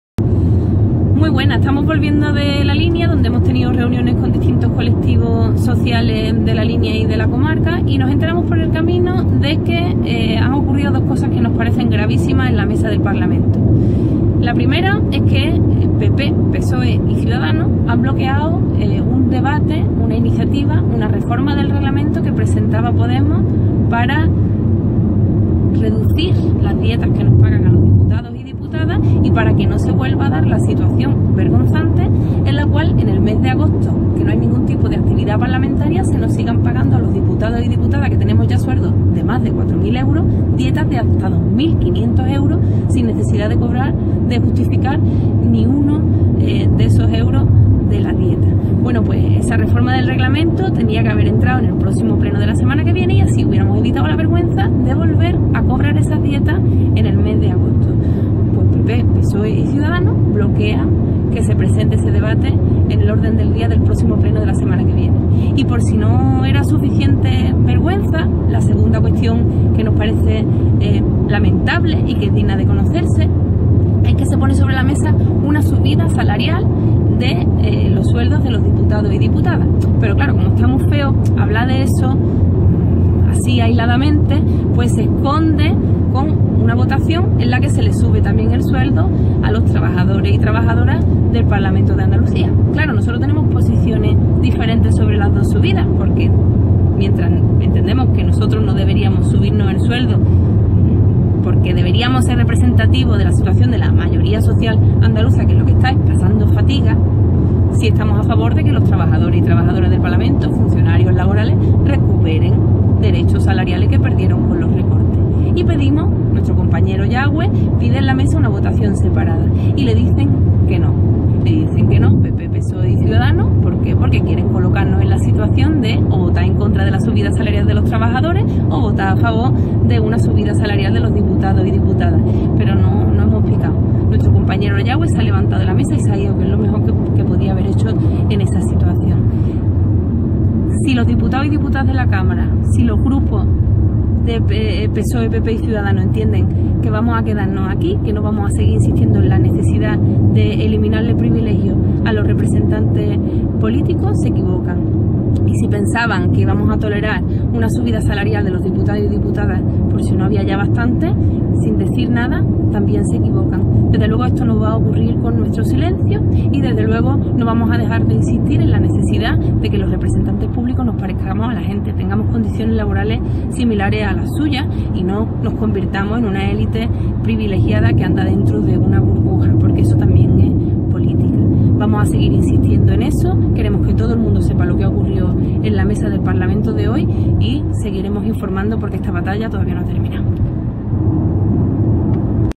Teresa Rodríguez denuncia un aumento de sueldos en el Parlamento
Teresa Rodríguez denuncia un aumento de sueldos de los diputados, "escondido de mala fe" en la subida a funcionarios